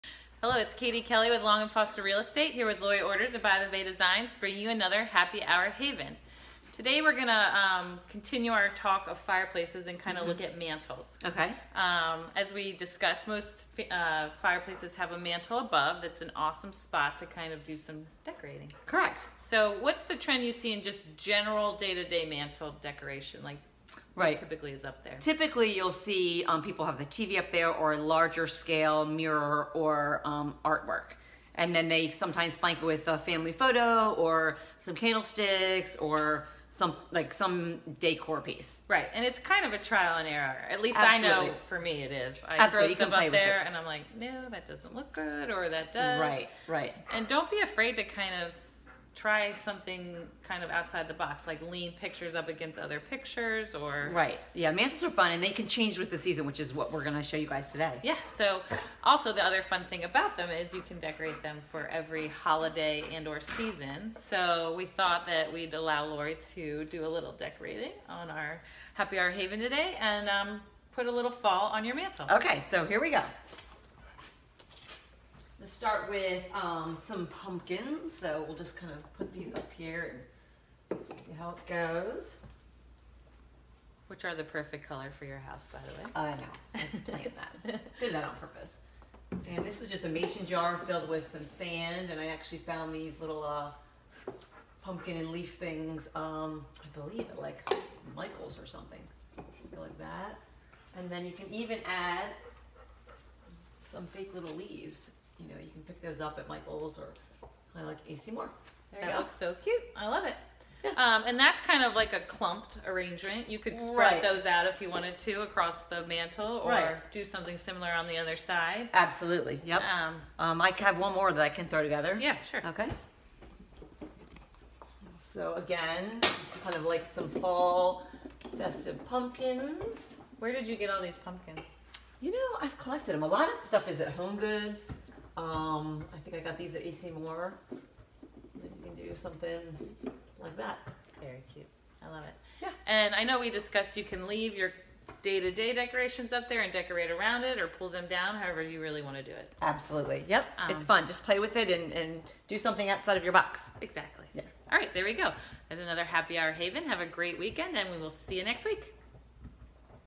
Happy Hour Haven #37...Mantle Decor
The mantle is the perfect place to add a little seasonal decor.  Quick tips and examples of some fall mantle decorating!